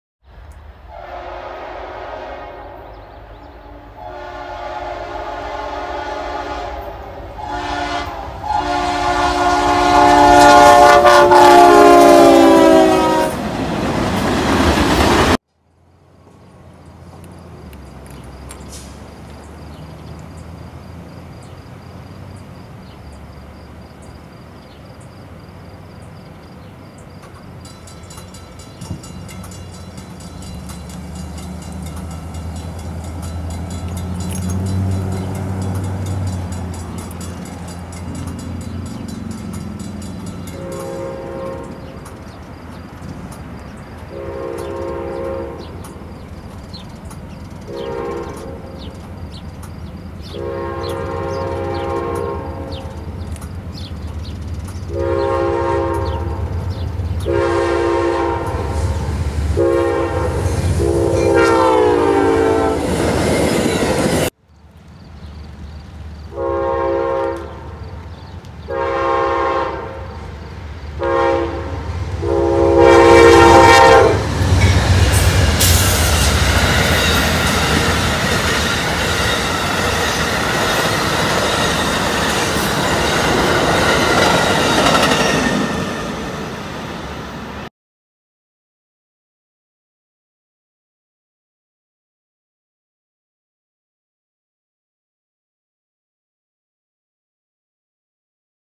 Amtrak Lake Shore Limited in Geneva, Ohio
Saturday July 13th at 8:11 AM Amtrak Lake Shore Limited #48 rounds the bend after a 2 hour delay.